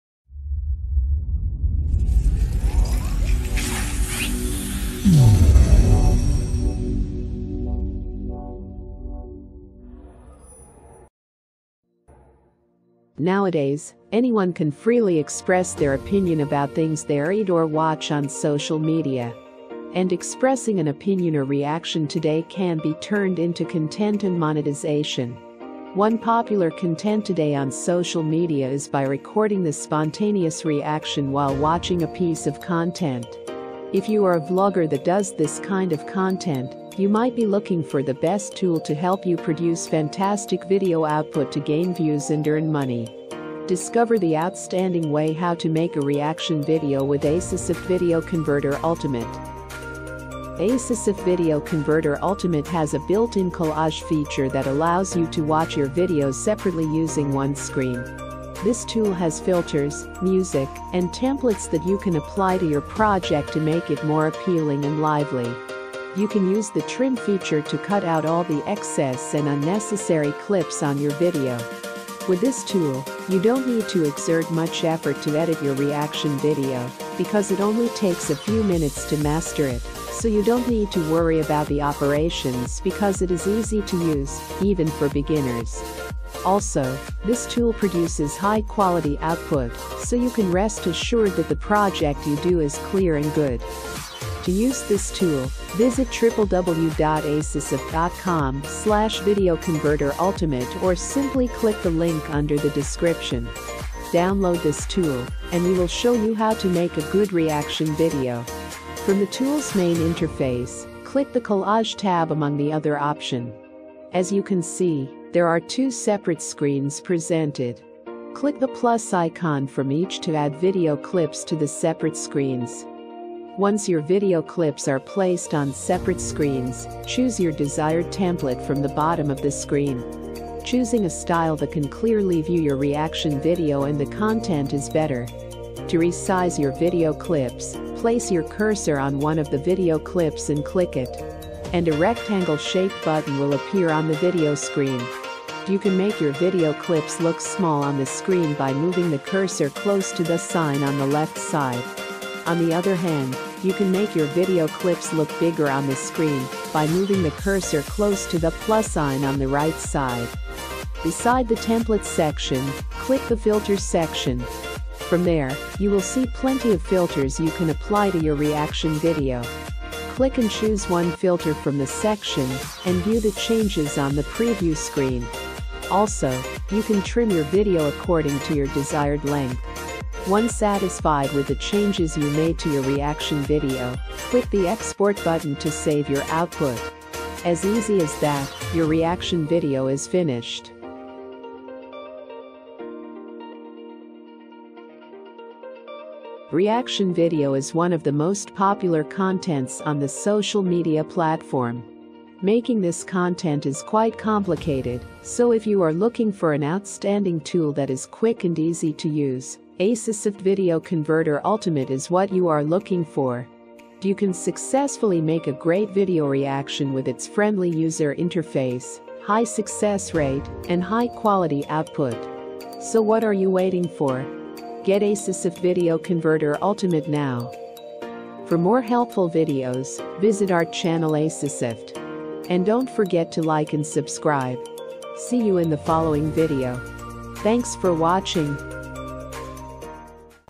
How to Make a Reaction Video
This wonderful video is a lead-in tutorial for users at an entry-level to make a reaction video.

You can capture people's reactions to songs, funny or sad clips, rap and so on while demonstrating the original songs at the same time here.

Watch and Learn all practical skills with our step-by-step instruction!